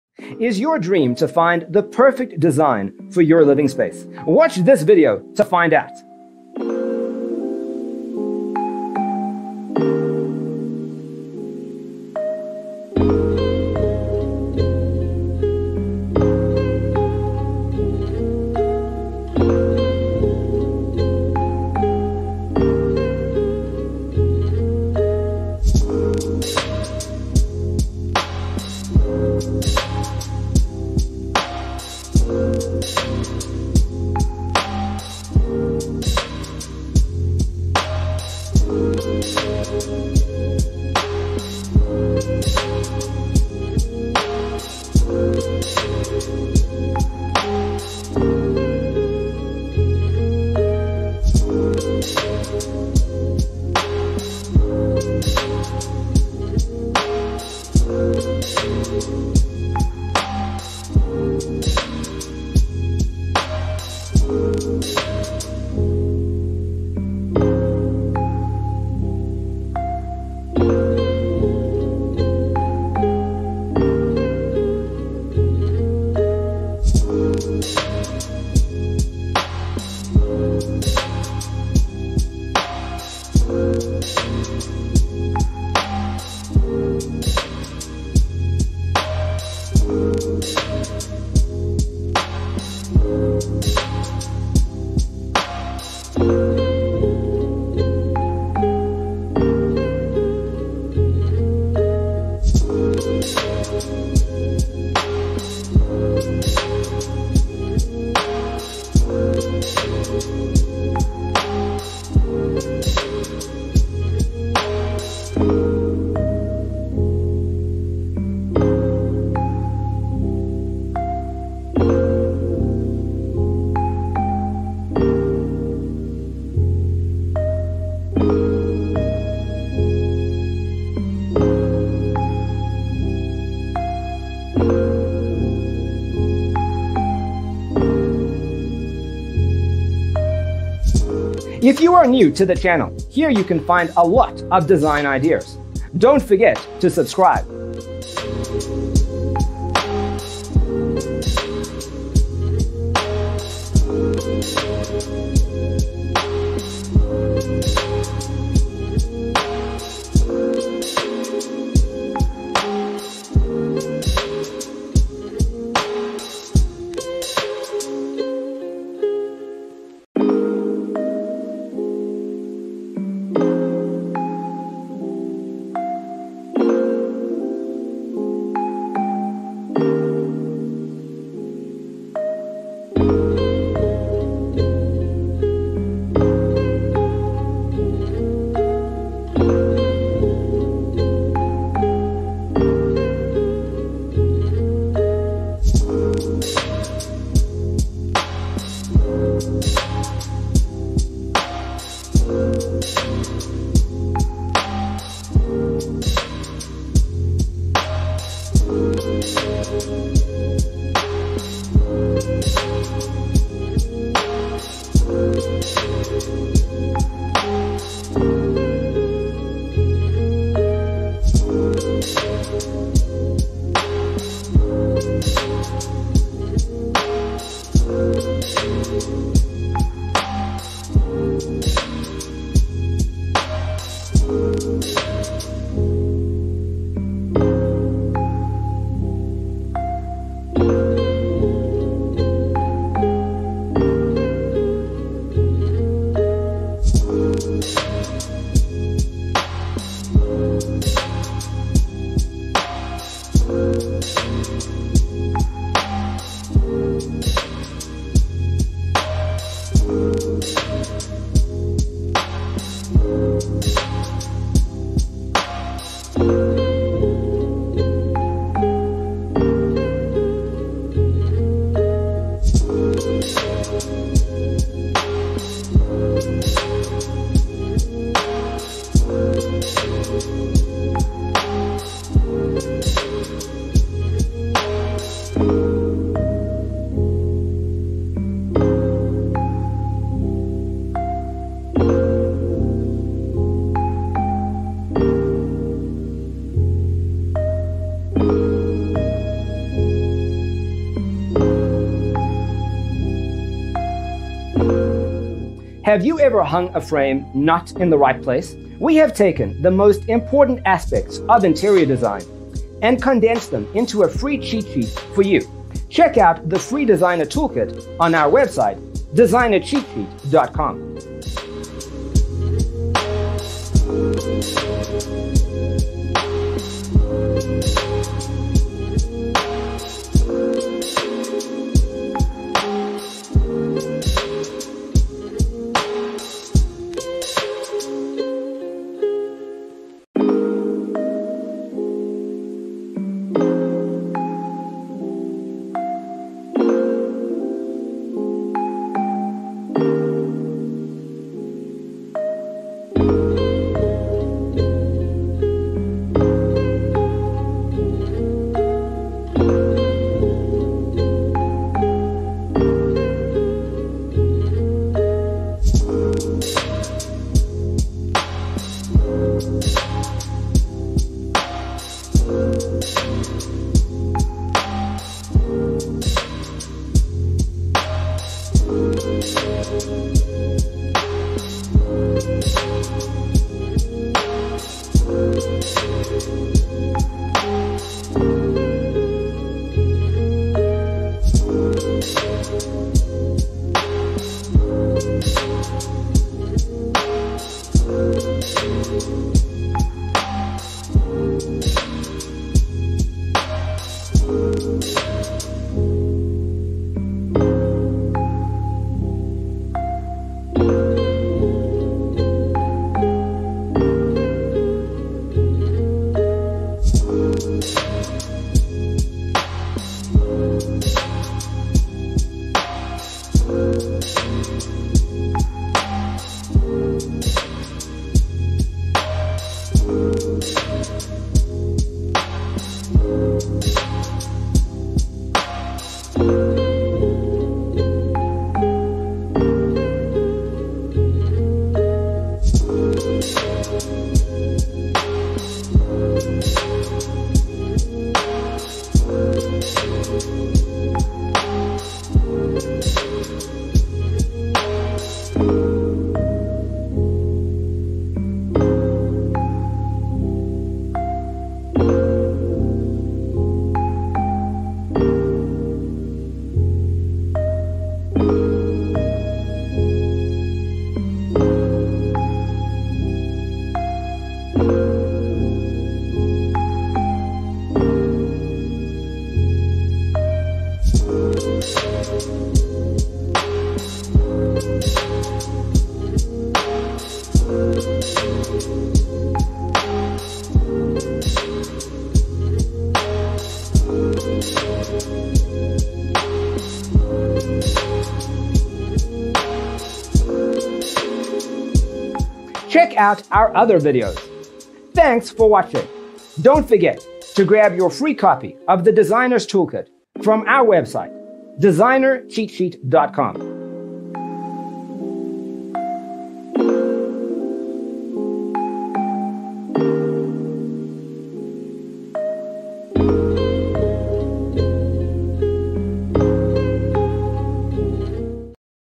75 Transitional Basement With A Two-sided Fireplace Design Ideas You'll Love 🌈
Browse transitional basement pictures. Discover a variety of transitional finished basement ideas, layouts and decor to inspire your remodel.

0:00:00 - Intro
0:00:12 - Transitional Basement With A Two-sided Fireplace Design Ideas You'll Love: Inspirations 1 to 25
0:03:03 - Transitional Basement With A Two-sided Fireplace Design Ideas You'll Love: Inspirations 26 to 50
0:06:07 - Transitional Basement With A Two-sided Fireplace Design Ideas You'll Love: Inspirations 51 to 75

Hello there!🌈